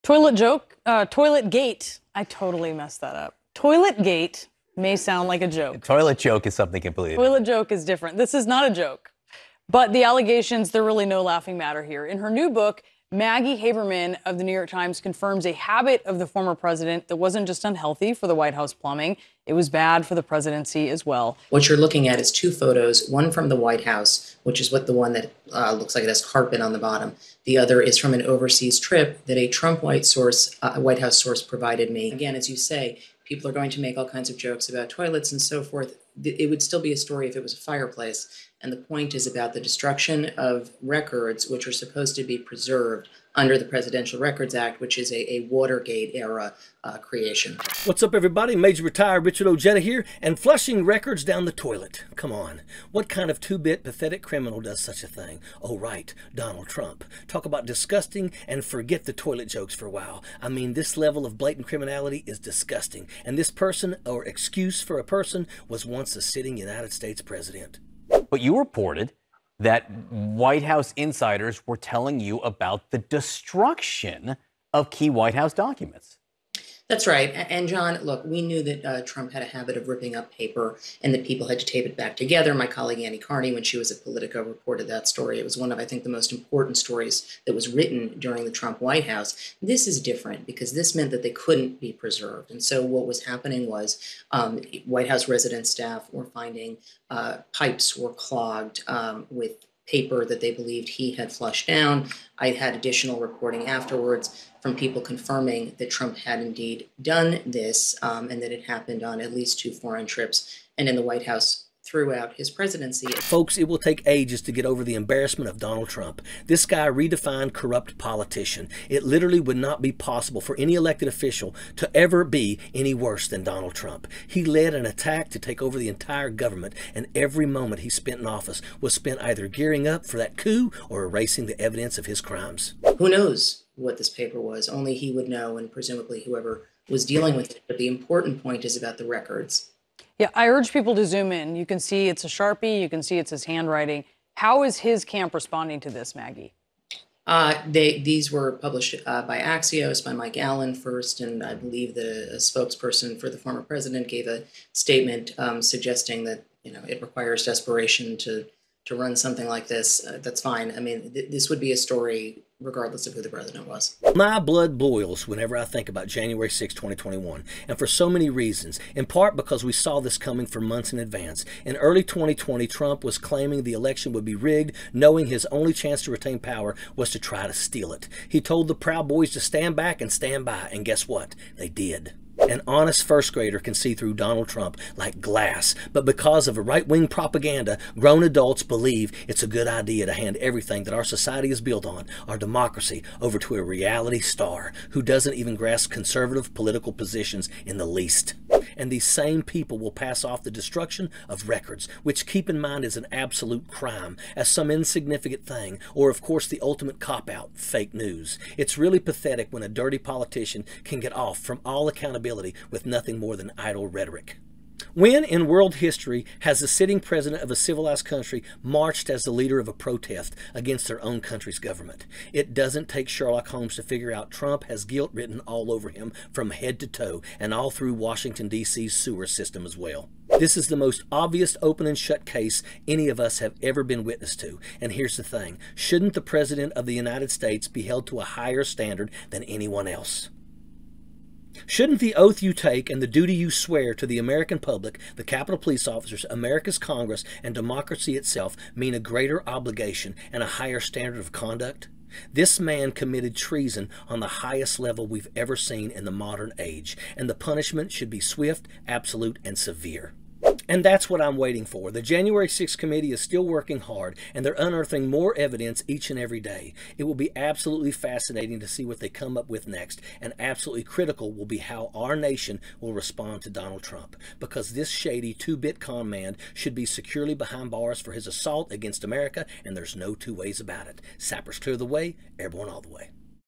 Trump CAUGHT Clogging White House Toilets
Donald Trump's nefarious method of disposing important documents has been revealed thanks to New York Times reporter Maggie Haberman. According to Haberman, and aided by photographic evidence, Trump had a habit of tearing apart documents and flushing them down White House toilets. Richard Ojeda breaks it down on Rebel HQ.